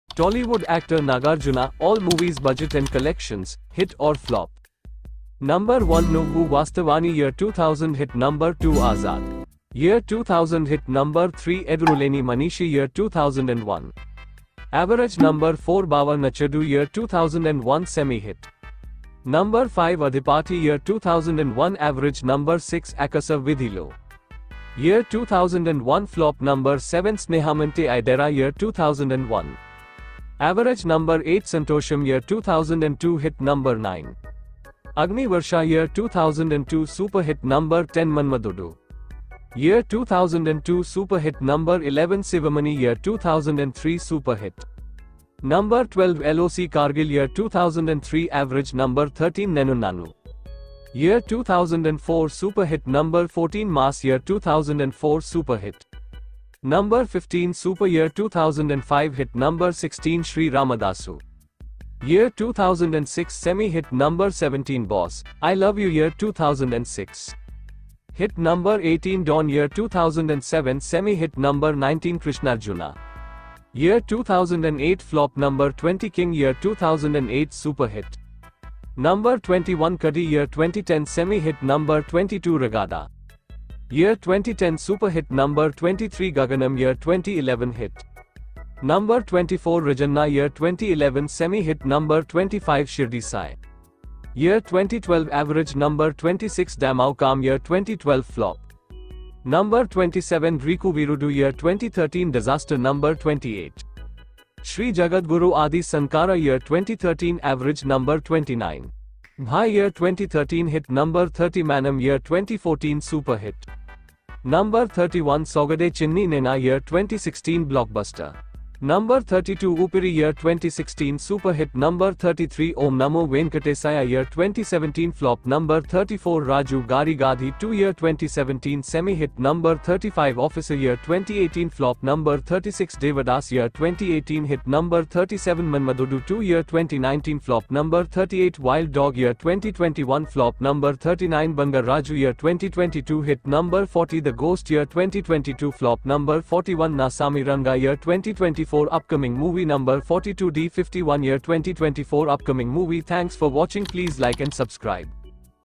Nagarjuna | All Movies Budget and Collections .(2000-2024) Hit or Flop
Nambar 1
Nuvvu Vastavani
Year 2000
Hit

Nambar 2
Azad
Year 2000
Hit

Nambar 3
Eduruleni Manishi
Year 2001
Average

Nambar 4
Bava Nachadu
Year 2001
Semi-hit

Nambar 5
Adhipathi
Year 2001
Average

Nambar 6
Akasa Veedhilo
Year 2001
Flop

Nambar 7
Snehamante Idera
Year 2001
Average

Nambar 8
Santosham
Year 2002
Hit

Nambar 9
Agni Varsha
Year 2002
Super-hit

Nambar 10
Manmadhudu
Year 2002
Super-hit

Nambar 11
Sivamani
Year 2003
Super-hit

Nambar 12
LOC Kargil
Year 2003
Average

Nambar 13
Nenunnanu
Year 2004
Super-hit

Nambar 14
Mass
Year 2004
Super-hit

Nambar 15
Super
Year 2005
Hit

Nambar 16
Sri Ramadasu
Year 2006
Semi-hit

Nambar 17
Boss - I Love You
Year 2006
Hit

Nambar 18
Don
Year 2007
Semi-hit

Nambar 19
Krishnarjuna
Year 2008
Flop

Nambar 20
King
Year 2008
Super-hit

Nambar 21
Kedi
Year 2010
Semi-hit

Nambar 22
Ragada
Year 2010
Super-hit

Nambar 23
Gaganam
Year 2011
Hit

Nambar 24
Rajanna
Year 2011
Semi-hit

Nambar 25
Shirdi Sai
Year 2012
Average

Nambar 26
Damarukam
Year 2012
Flop

Nambar 27
Greeku Veerudu
Year 2013
Disaster

Nambar 28
Sri Jagadguru Aadi Sankara
Year 2013
Average

Nambar 29
Bhai
Year 2013
Hit

Nambar 30
Manam
Year 2014
Super-hit

Nambar 31
Soggade Chinni Nayana
Year 2016
Blockbuster

Nambar 32
Oopiri
Year 2016
Super-hit

Nambar 33
Om Namo Venkatesaya
Year 2017
Flop

Nambar 34
Raju Gari Gadhi 2
Year 2017
Semi-hit

Nambar 35
Officer
Year 2018
Flop

Nambar 36
Devadas
Year 2018
Hit

Nambar 37
Manmadhudu 2
Year 2019
Flop

Nambar 38
Wild Dog
Year 2021
Flop

Nambar 39
Bangarraju
Year 2022
Hit

Nambar 40
The Ghost
Year 2022
Flop

Nambar 41
Naa Saami Ranga
Year 2024
Upcoming movie

Nambar 42
D51
Year 2024
Upcoming movie

Please